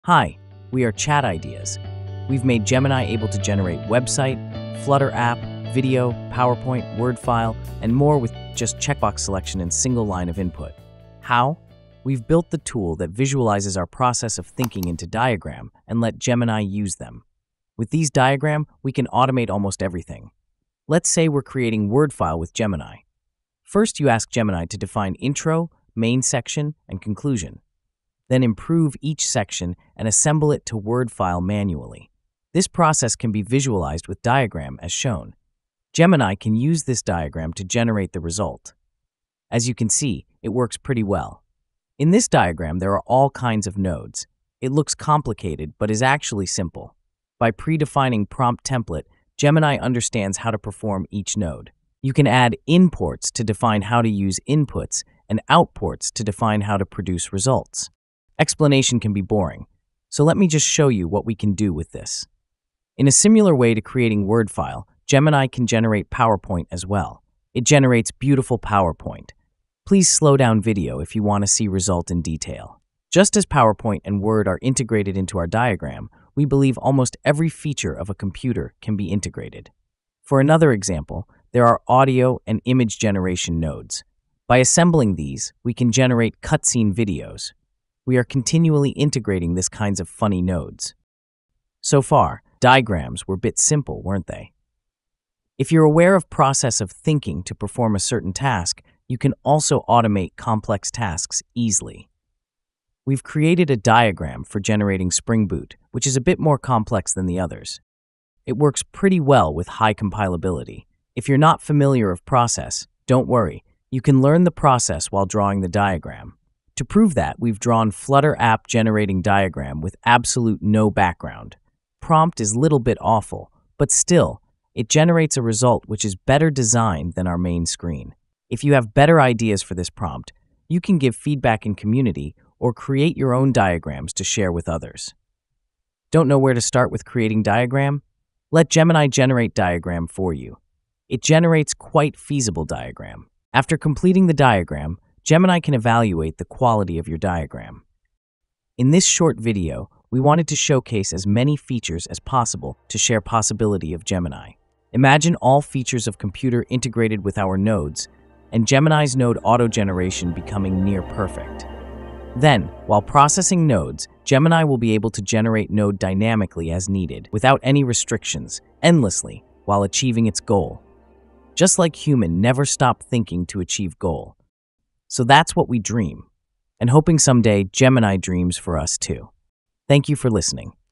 Generate Everything with a Single Line of Input
1. How to Use
Simply login and write whatever you like.
Since Auto Pilot prompt is now available, selecting checkbox is no longer necessary.

2. Available Features
0:20 - Word file
0:58 - PowerPoint
1:20 - Cutscene Video
1:35 - Spring Boot
1:57 - Flutter

3. How to Draw Diagram
This part isn't necessary if you plan to use our predefined diagrams.
However, if you want to create your own diagram, I recommend starting with the cloning Word generation diagram.
Since the Word generation diagram includes many basic diagram features, it will enhance your understanding of our diagram.
Once you're familiar with that, you'll be able to define any diagram you desire and use it by selecting the checkbox.

We can provide diagram for any kind of automation.